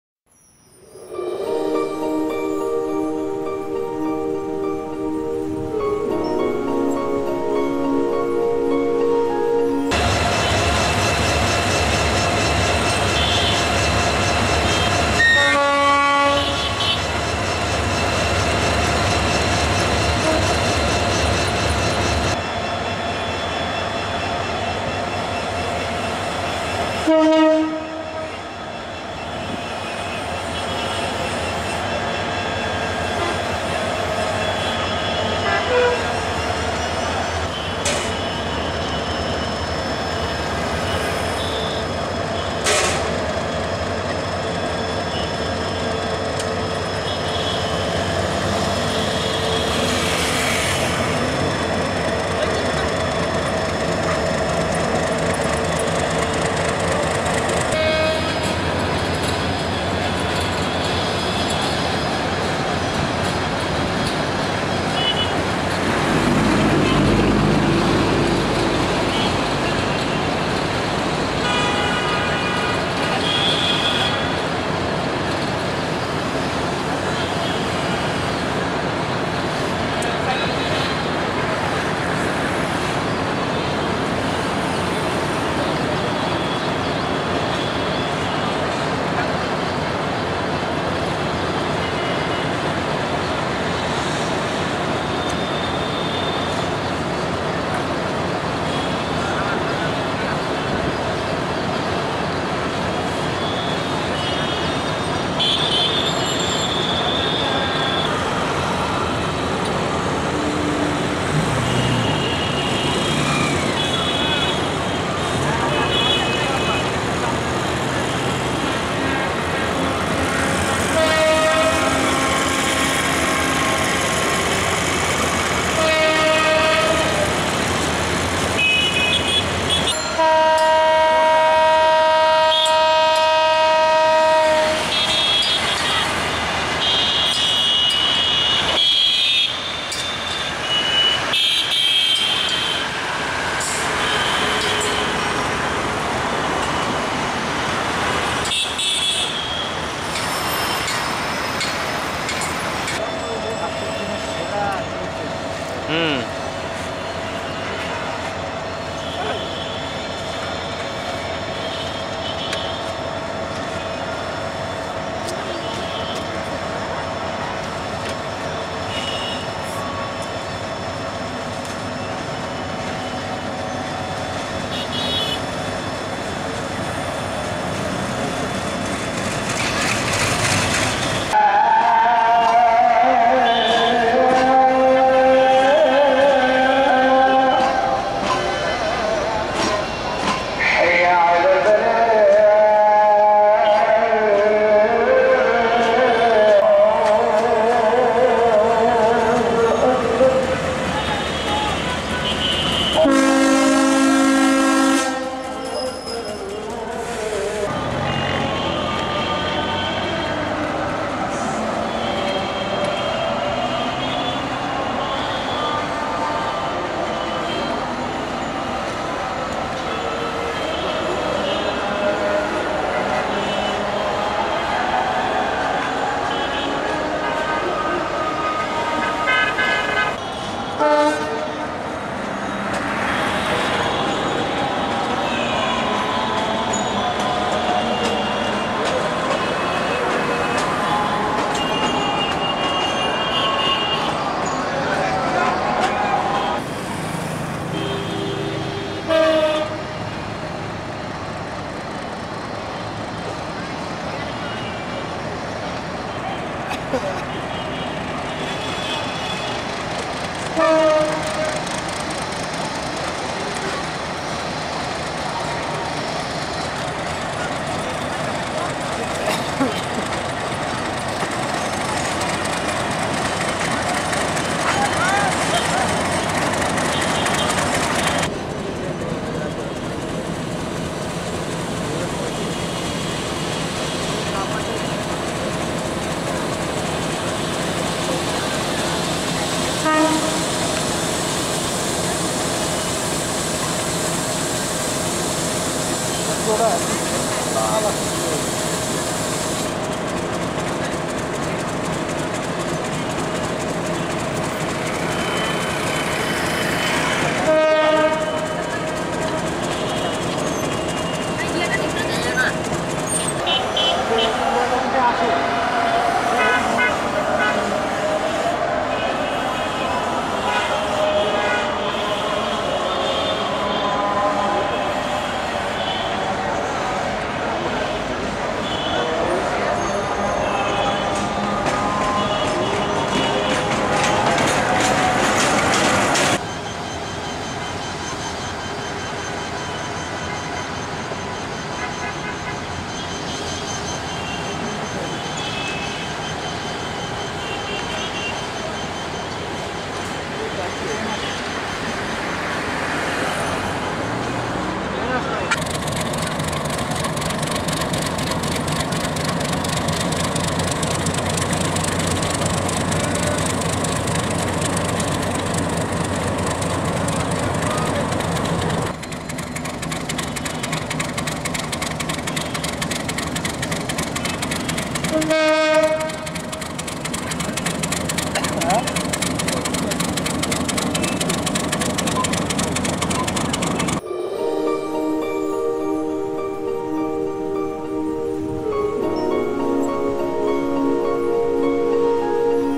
All Types of Diesel Locomotives (ALCO || EMD) in Bangladesh Railways
Location: Kamlapur Railway Station.Dhaka